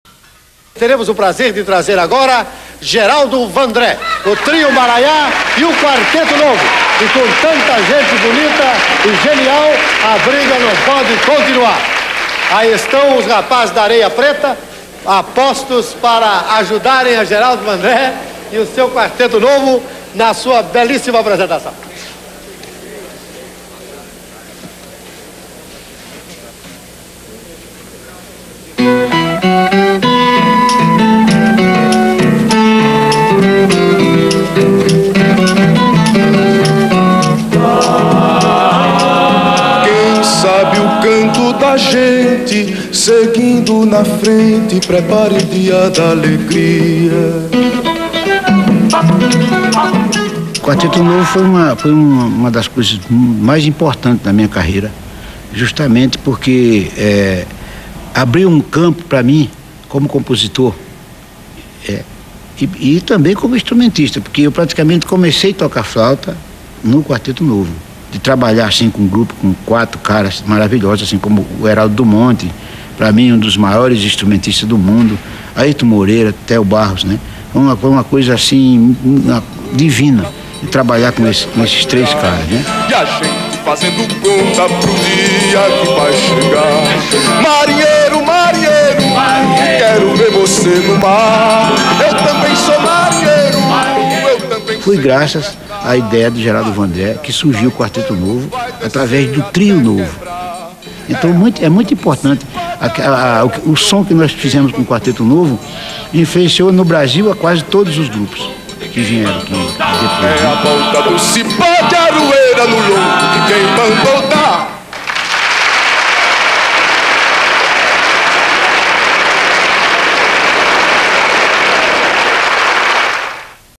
Quarteto Novo, Geraldo Vandré - Hermeto Heraldo Théo & Airto
Vídeo raro (talvez uma das filmagens mais antigas do Hermeto, de cabelo curto e sem barba) do legendário Quarteto Novo acompanhando Geraldo Vandré junto do Trio Marayá.
Rare footage (maybe the earliest known, with a short haired and beardless Hermeto) from the legendary Quarteto Novo combo supporting singer Geraldo Vandré with Trio Marayá. Enjoy!